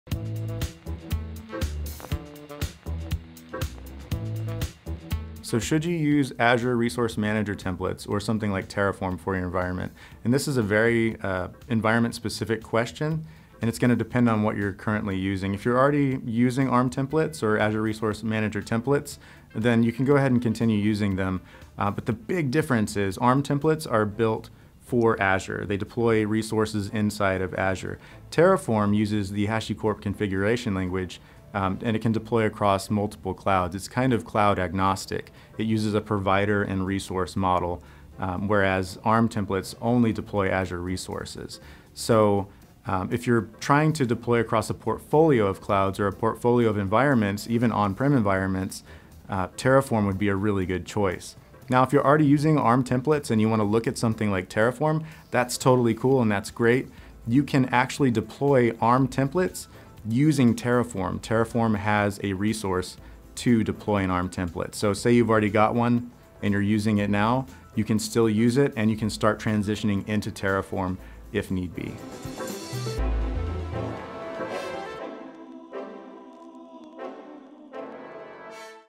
Should I use ARM Templates or something like Terraform for my environment? | One Ops Question
We are back answering your question about operation in a cloud\hybrid environment.  This week, Zachary Deptawa answers a question we've been getting a lot lately.

"Should I use Azure Resource Manager Templates or something like Terraform for my environment?"